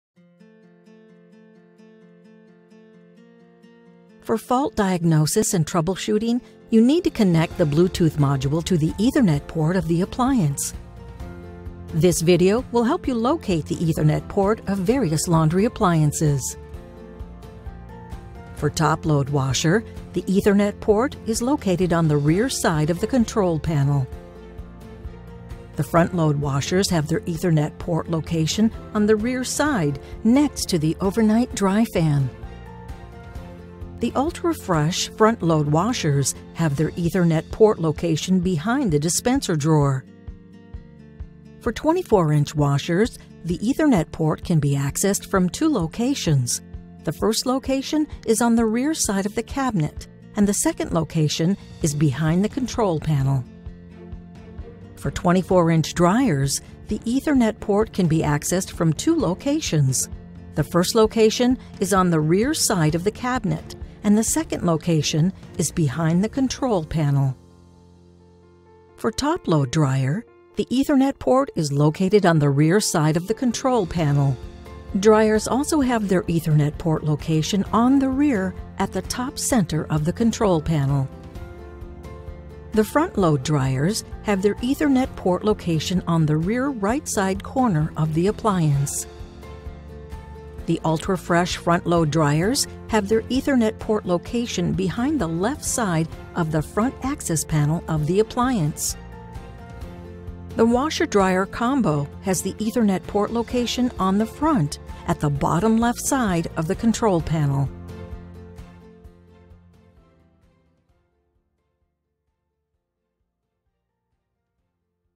Ethernet Port Location of Various Laundry Appliances
This video will help you to locate the Ethernet Port of various laundry appliances.